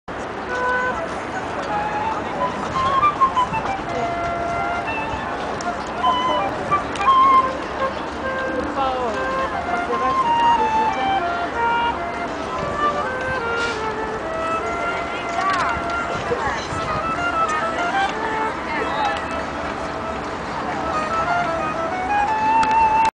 Relaxing Flute Music on Paris Bridge
A pan flute plays while panning the camera near a panhandler in paris france